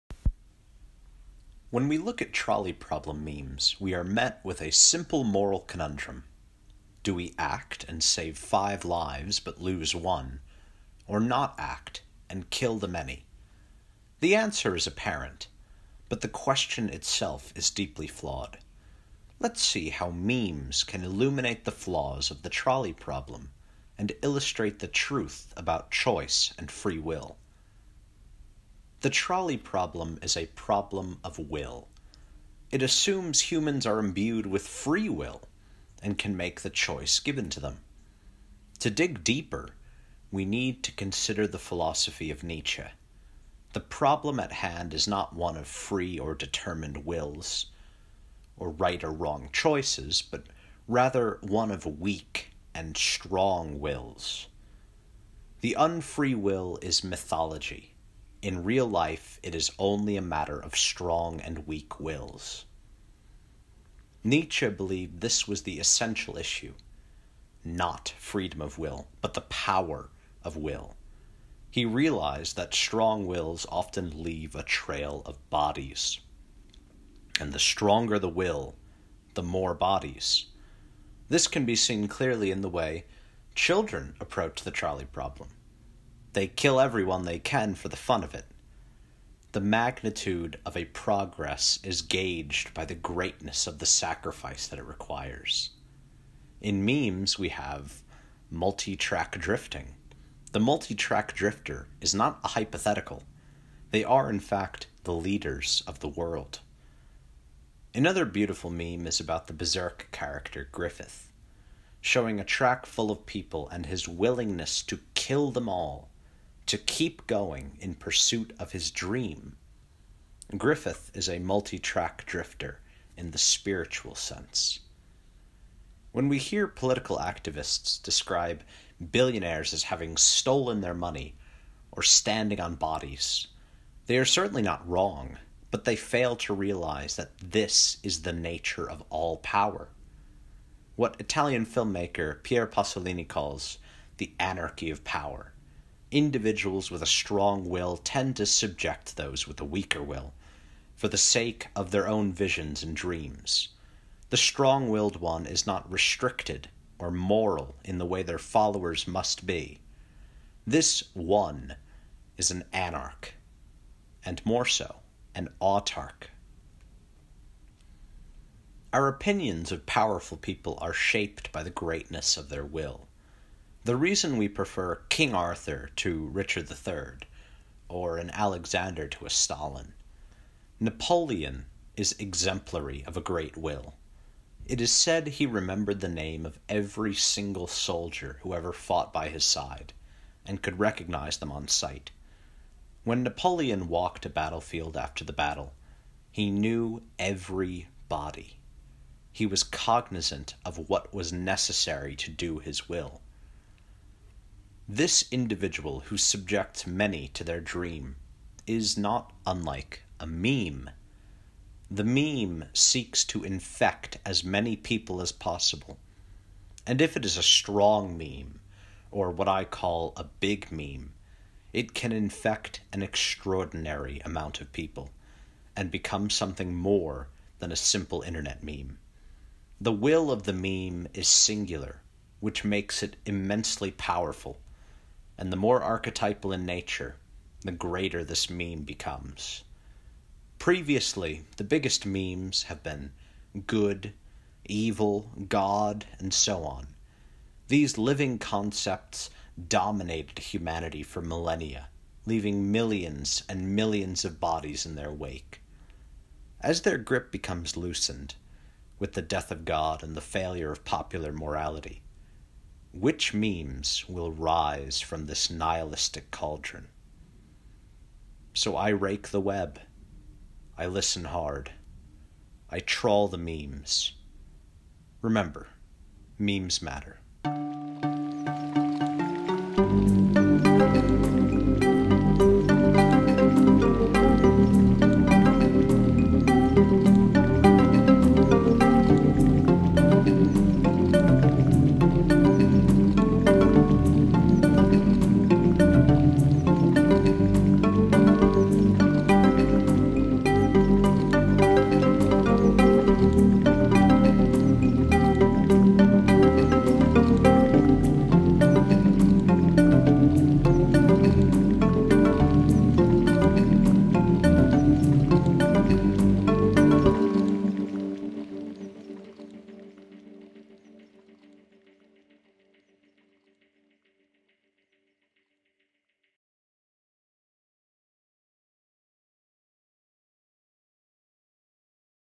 Trolley Problem Meme Analysis: Philosophy of Will
A trolley problem meme analysis, one of the most popular philosophy memes and how it relates to Nietzsche's Will.
Reading List
Comics: @aeoniccomics on instagram